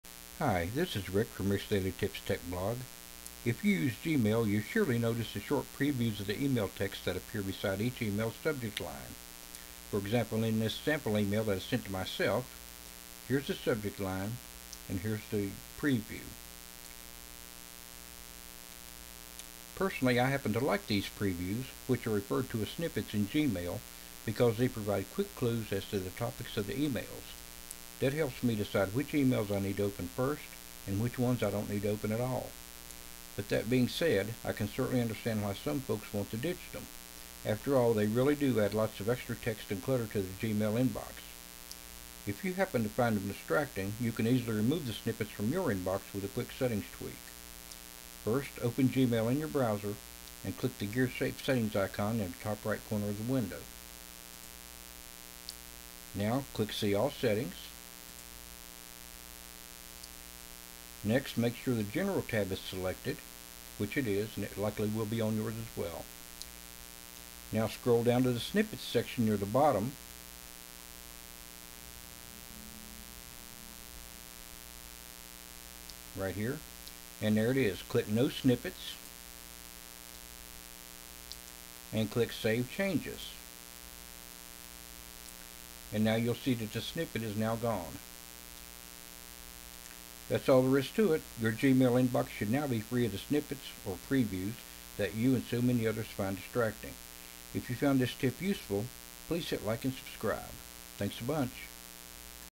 How to remove the snippets (previews) from your Gmail Inbox
As you probably know, Google places short previews (called snippets) beside each Gmail message to give you a clue as to the content of each email before you actually open it.

I happen to like those snippet previews, but plenty of folks find them distracting. If you're one of them you can easily remove the snippets from your Gmail Inbox with a quick settings tweak. This video shows how, step-by-step.